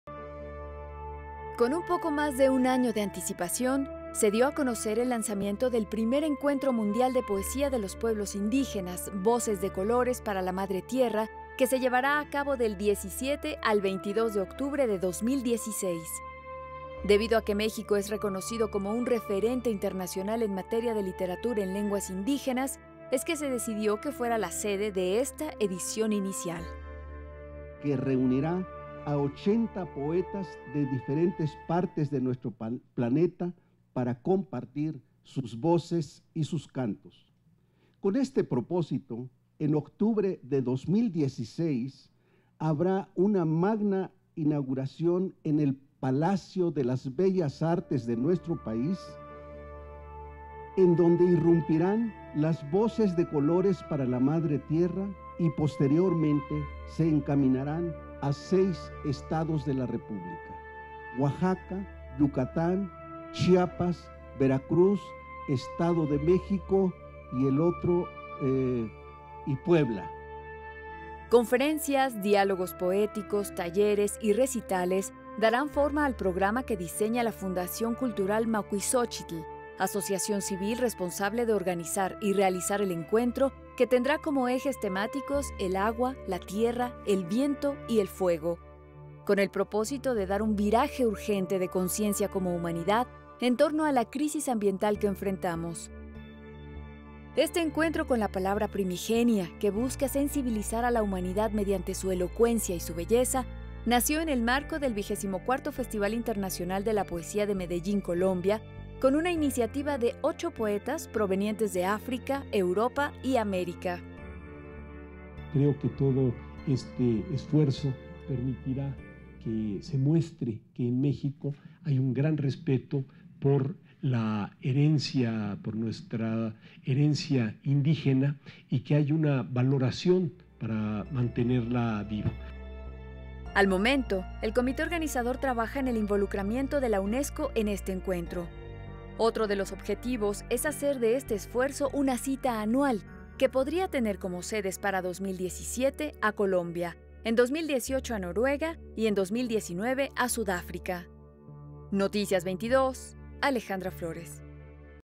1er Encuentro Mundial de Poesía de los Pueblos Indígenas
Este primer encuentro lleva por título "Voces de colores para la madre Tierra" y se llevará a cabo del 17 al 22 de octubre del 2016 en la Ciudad de México
Por: Alejandra Flores